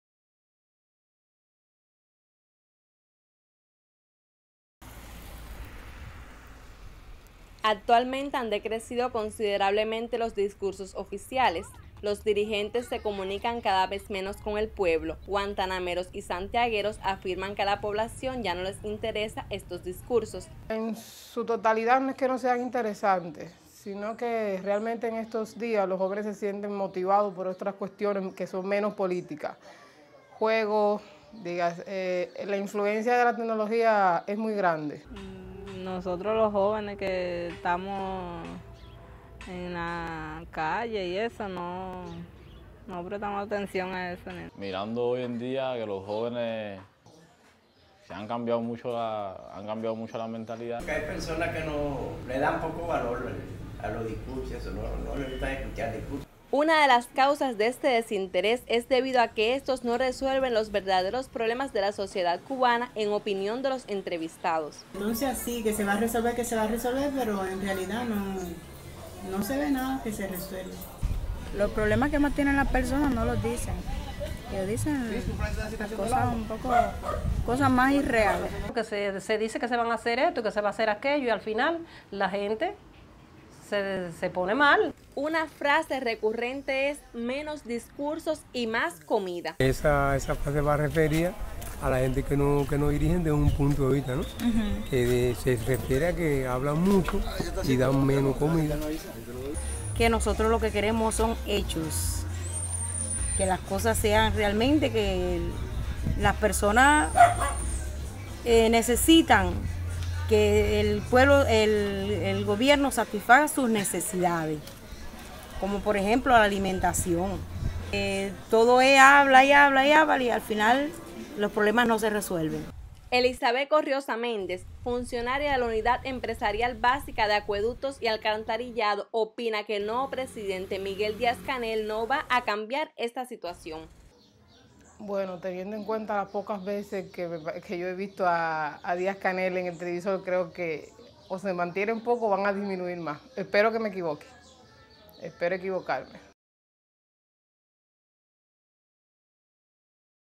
Discursos Oficiales
Mas de lo Mismo. Asi diria un buen cubano; pero ya cuba tenemos "El Paquete". ya no son solo Dos canales de Televisión. La realidad es que el cubano se divorció hace mucho de el acontecer socio-político de la nación. Tendremos el despertar que muchos anhelamos??